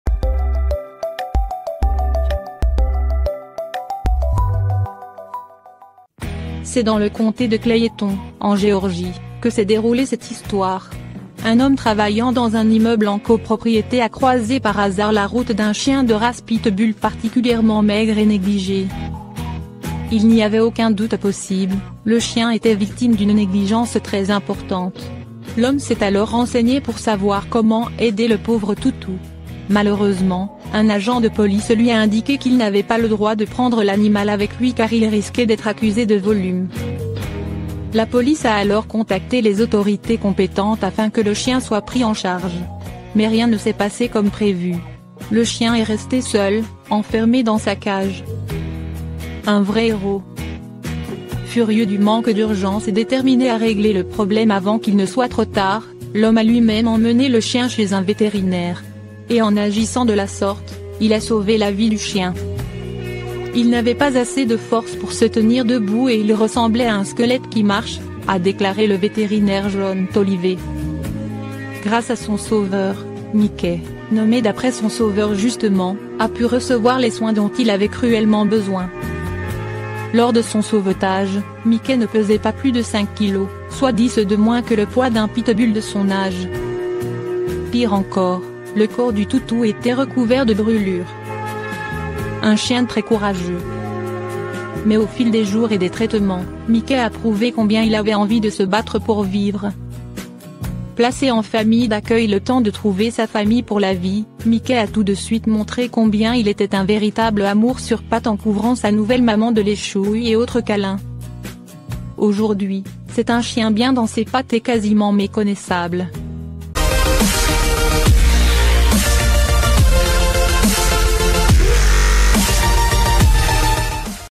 Il sauve un Pitbull qui ne pèse que 5 kilos, quelques semaines plus tard il éclate en sanglot en le
Le pauvre Mikey avait besoin d’aide.
C’est dans le comté de Clayton, en Géorgie, que s’est déroulée cette histoire. Un homme travaillant dans un immeuble en copropriété a croisé par hasard la route d’un chien de race Pitbullparticulièrement maigre et négligé.